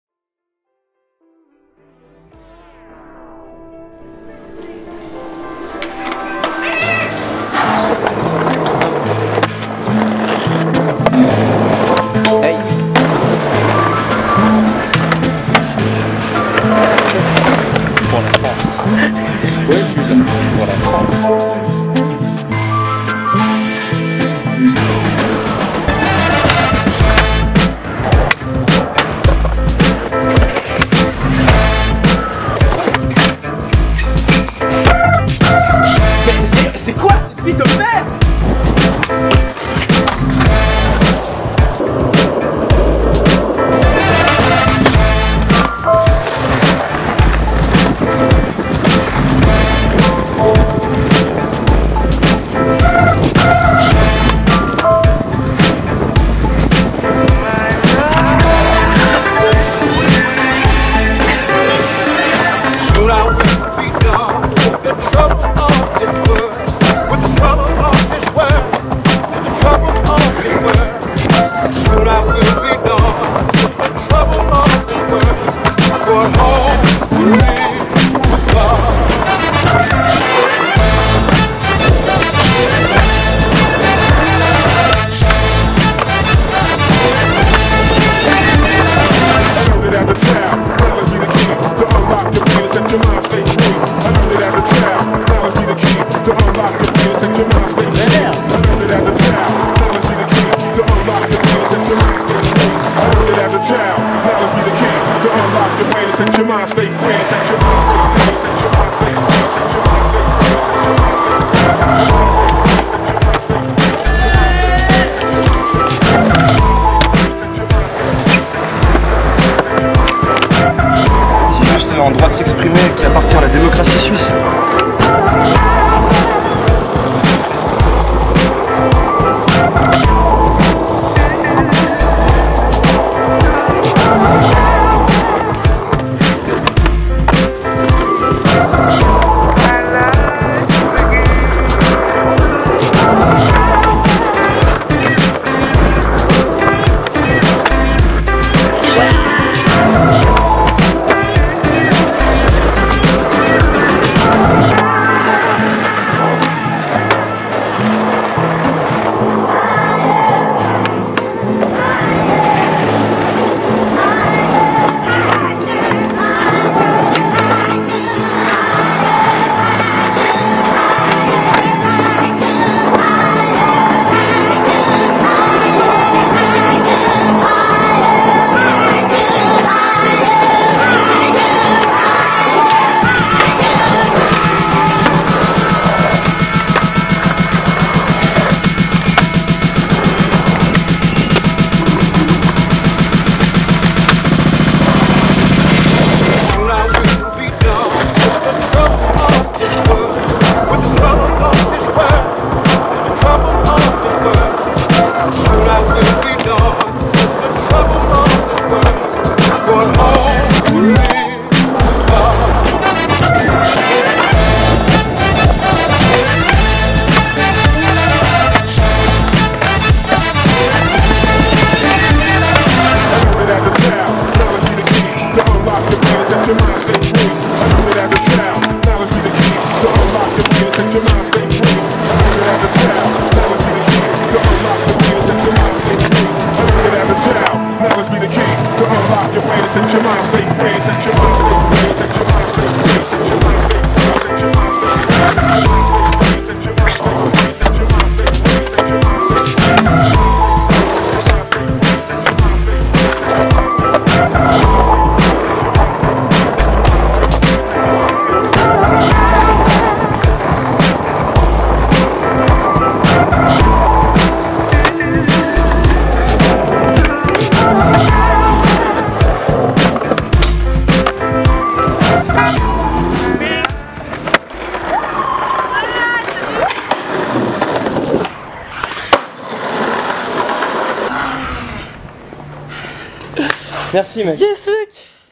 Madreams - Spots Are Everywhere
Session skate à Genève juste pour le plaisir!
Les spots sont partout. edited by Steffen Christen.
Featuring:
Romain Grobéty, Menelik Jourdan, Jasper McDowell, Mathieu LDG, Steffen "Jesus" Christen, Simon Perrotet, Hugo Malhao, Cyril Jubin, Steven Jean, Antoine Rauber, Alexander Rademaker, Greg Artisi, Luca Ostinelli et Mikael Lewertoff.